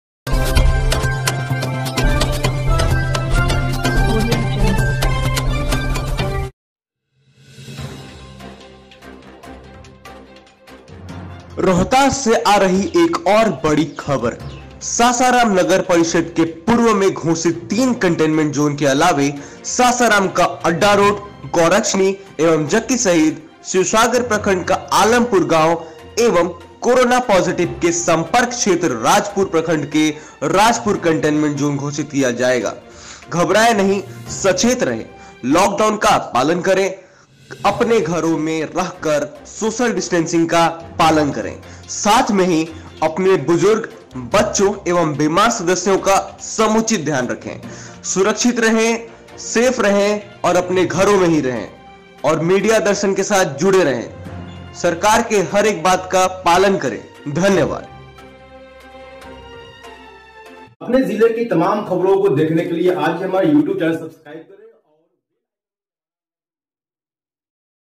रोहतास में 5 नए कंटेन्मेंट जोन घोषित किया जाएगा || CORONA IN ROHTAS || SASARAM ME CORONA
सासाराम नगर परिषद के पूर्व में घोषित तीन कंटेनमेंट जोन के अलावे सासाराम का अड्डा रोड, गौरक्षणी एवं जक्की शहीद, शिवसागर प्रखंड का आलमपुर गांव एवं कोरोना पॉजिटिव के संपर्क क्षेत्र राजपुर प्रखंड के राजपुर को कंटेनमेंट जोन घोषित किया जायेगा. घबराएं नहीं. सचेत रहे. लॉकडाउन का पालन करें. अपने घरों में रहकर सोशल डिस्टेंस का पालन करें. साथ ही अपने बुजुर्ग एवं बीमार सदस्यों का समुचित ध्यान रखे.